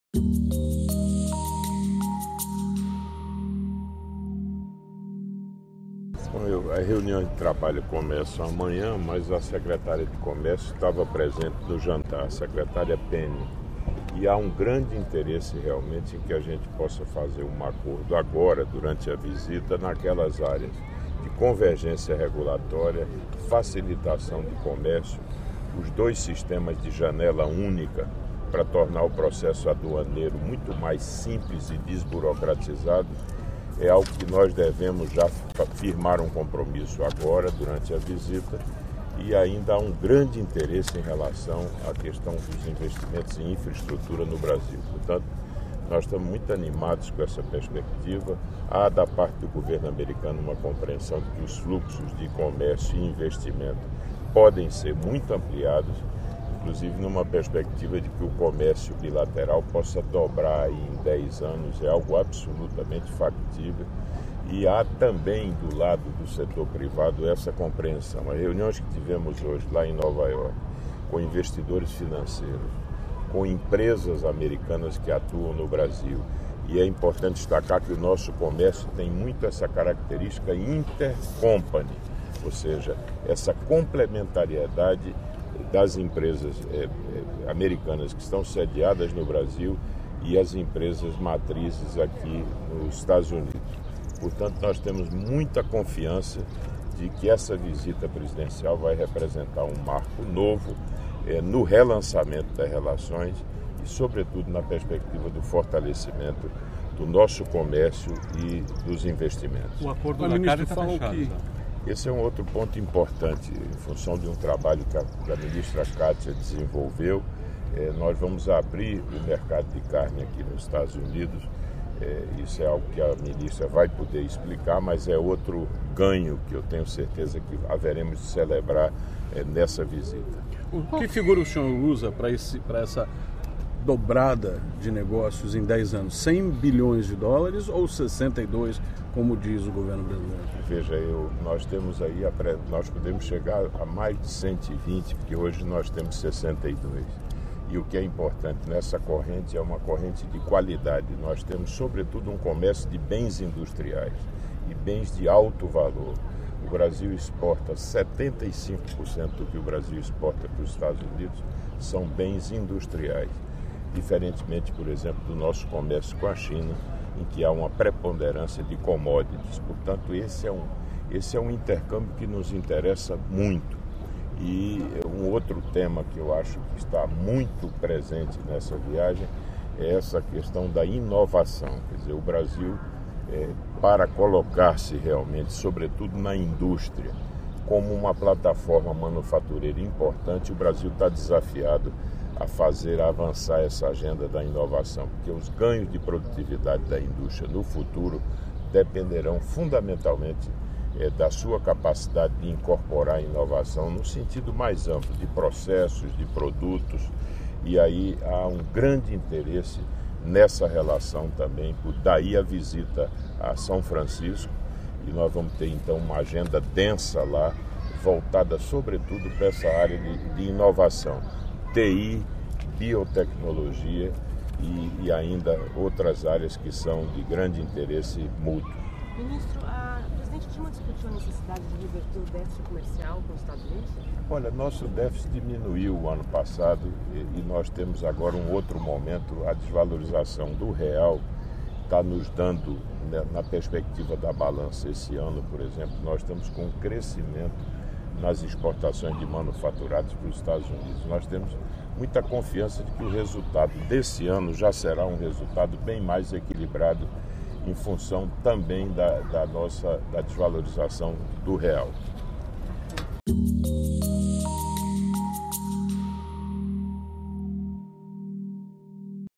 Brasil e EUA fortalecem relação na área de comércio e investimentos
ATIVIDADES DA PRESIDENTA - 29.06.15: Representantes de Brasil e Estados Unidos se encontraram, nesta segunda-feira (29), durante jantar privado na Casa Branca, em Washington. Em entrevista, o ministro do Desenvolvimento, Indústria e Comércio Exterior, Armando Monteiro, disse que a visita vai fortalecer as relações comerciais entre Brasil e EUA. Monteiro afirmou que há um grande interesse de ambas as nações para realizar acordos nas áreas de convergência regulatória, facilitação de comércio, simplificação do processo aduaneiro, e investimentos em infraestrutura e logística.